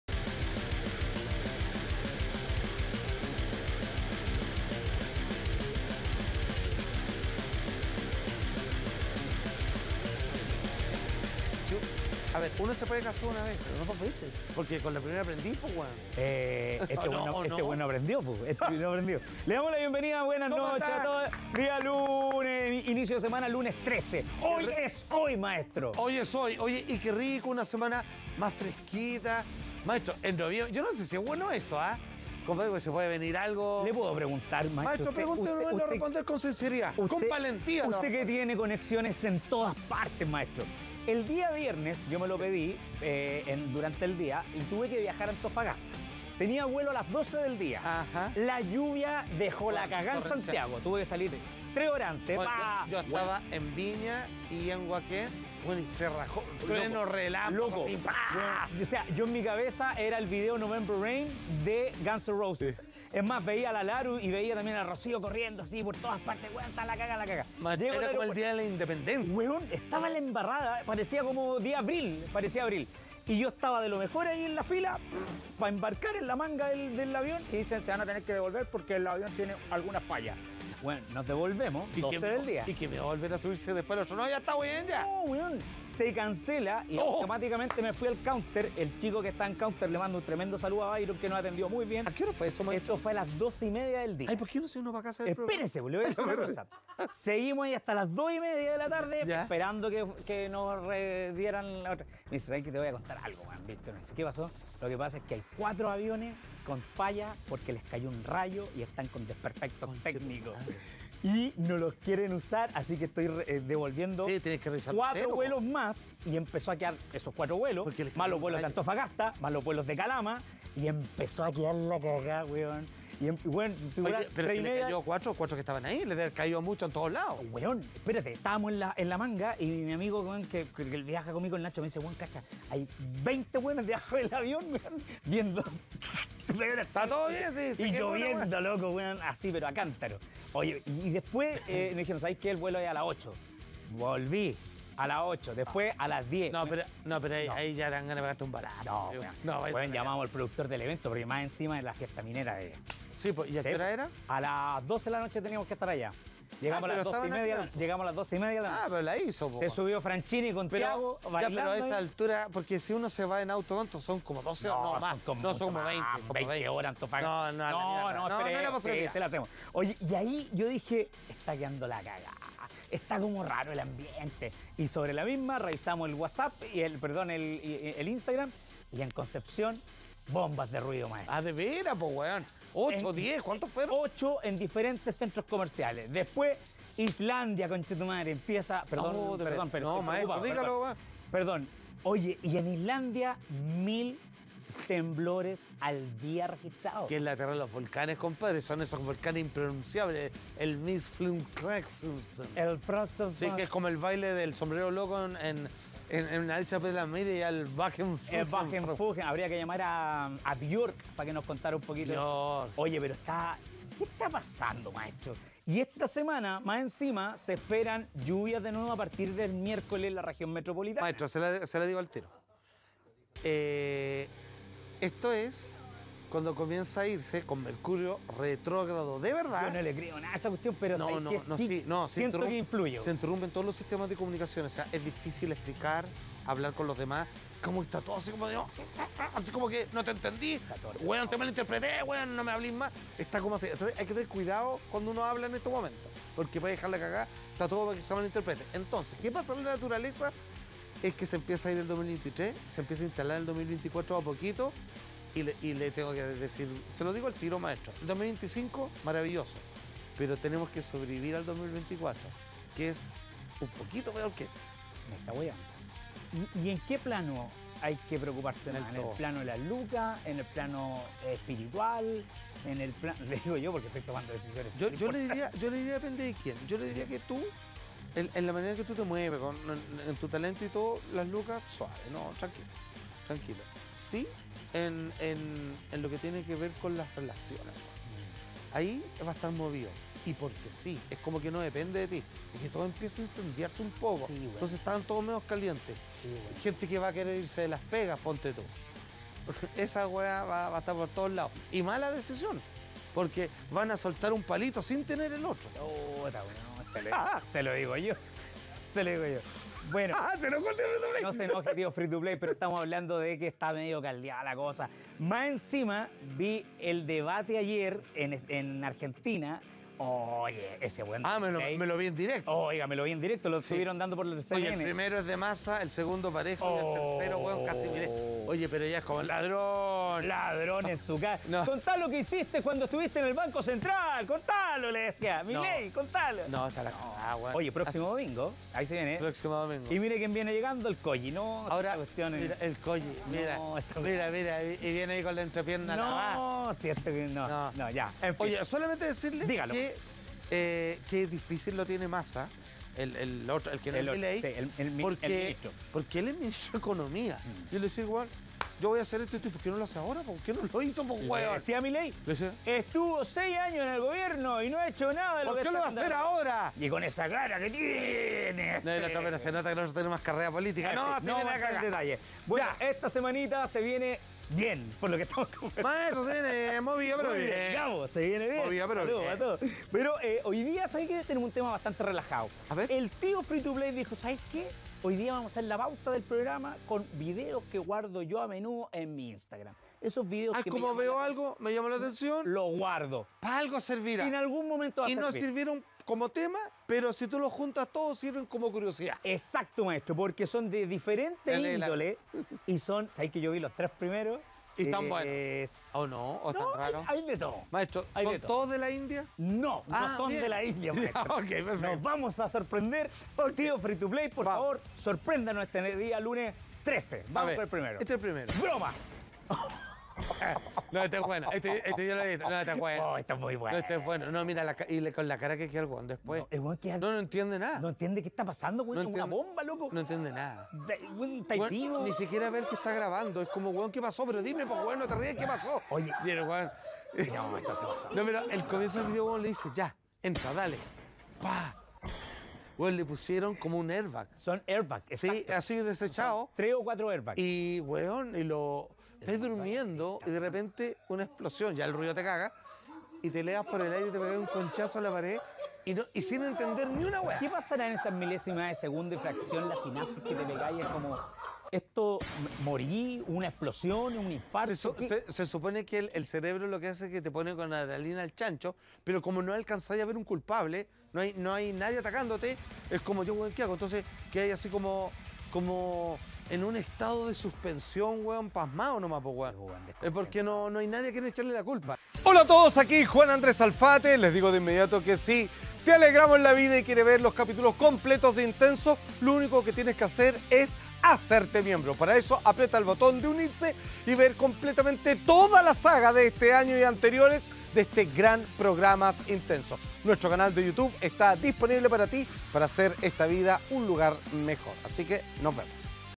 ¡ESPECIAL VIDEOS GUARDADOS DEL TÍO INTENSOS!
Comenzamos la semana revisando los videos guardados del tío Free to Play. Junto al maestro Salfate y Víctor, comentarán diferentes videos absurdos que suben las personas a sus redes sociales, desde duchas humanas automáticas hasta accidentes antes de tirarse piqueros en el mar.

Hazte miembro del canal para ver los capítulos completos 🤩

¡También puedes seguirnos en nuestras redes! 😍